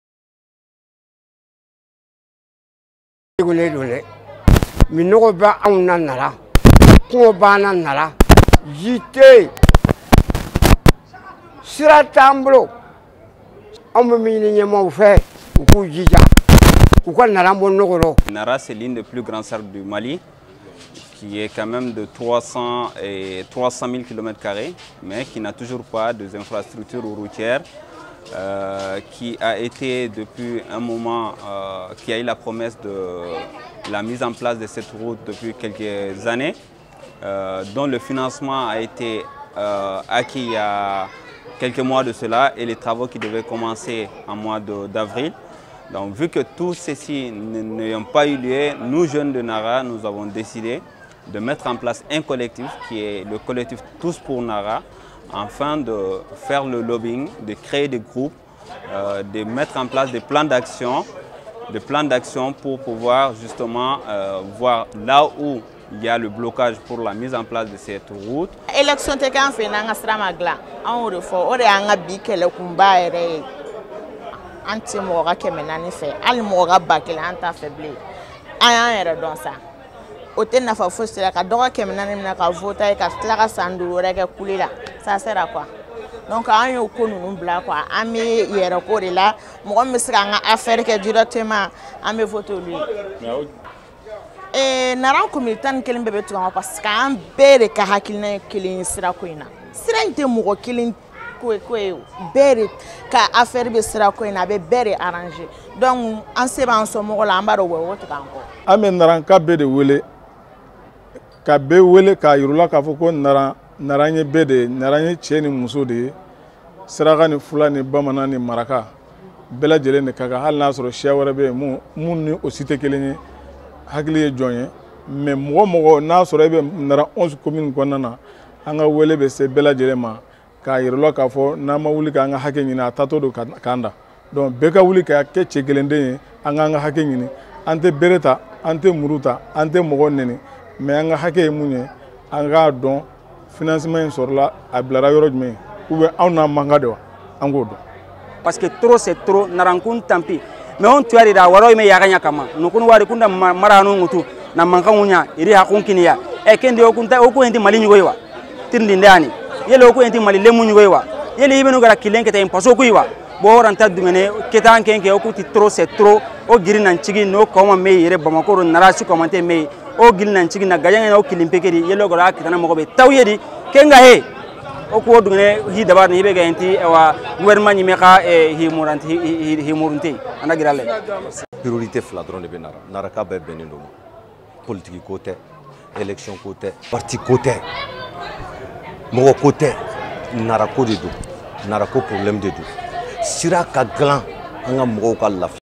2ème assemblée généla jeunesse de Nara s'engage pour la route Kwala Nara
La jeunesse de Nara a tenu ce jeudi 3 mai sa deuxième assemblee générale pour exiger le démarrage des travaux de bitumage de la route Nara - Kwala et des mesures immédiates contre l'insécurité.
Regardez la vidéo. crédit MalikibarouTv